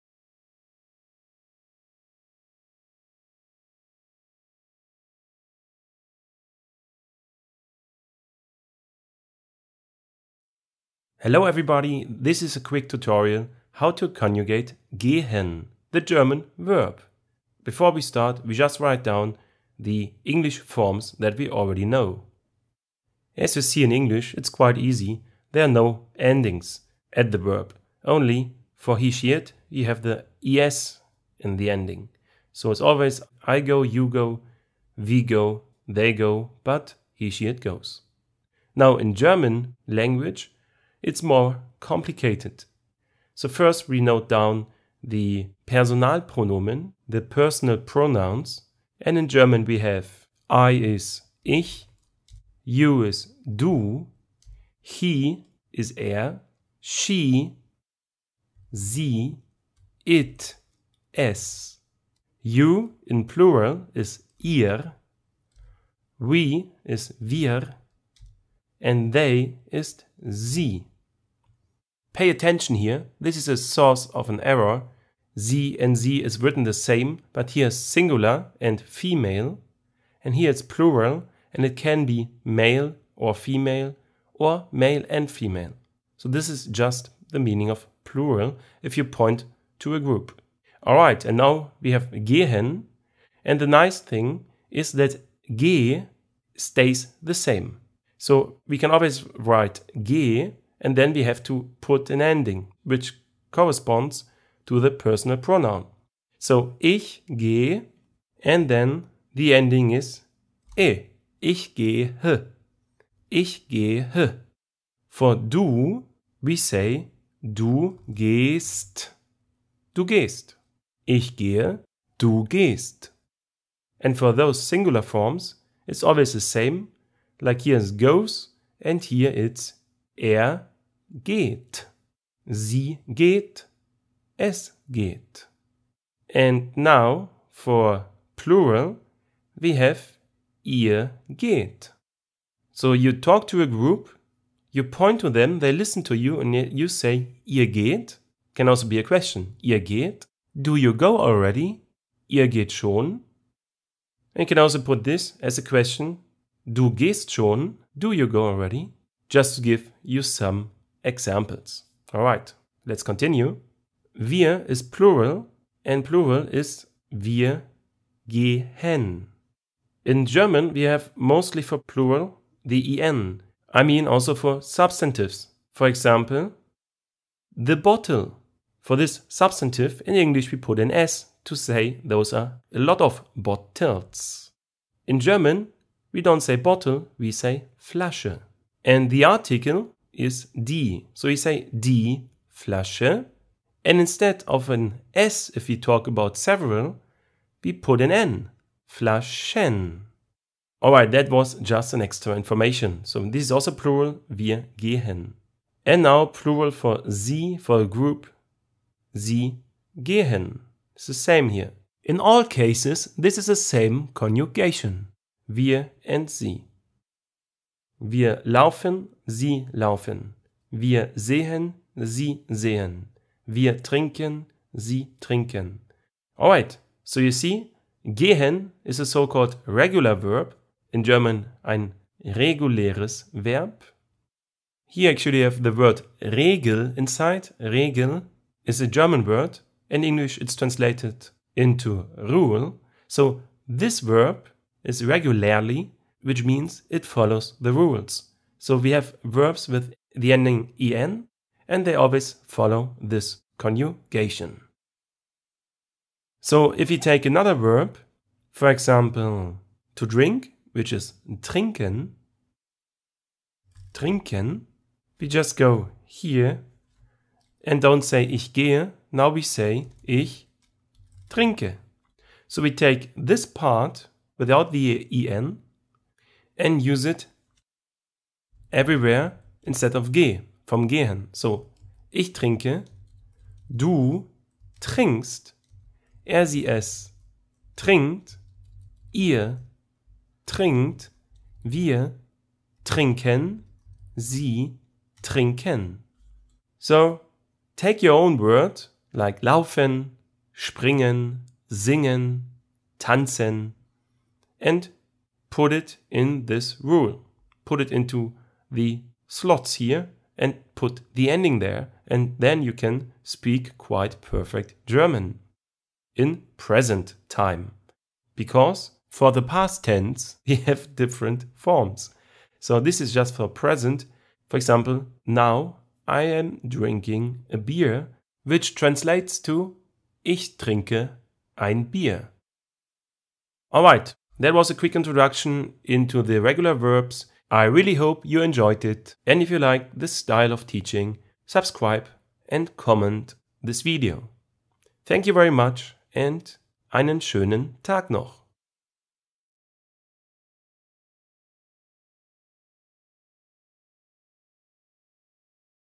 Learn German How to conjugate regular verbs such as 'gehen'
Here you learn in 6 min how to conjugate regular verbs in German language ("Reguläre Verben"). We also look to personal pronouns quickly ("Personalpronomen").

A German course for foreigners that can speak English / beginner level. Or just let's say "Deutsch für Ausländer".

I am a native German speaker, enjoy =)

Have fun, learn a language and enjoy life!

You can copy, share, edit the video as you like. No copyright. But please don't sell it.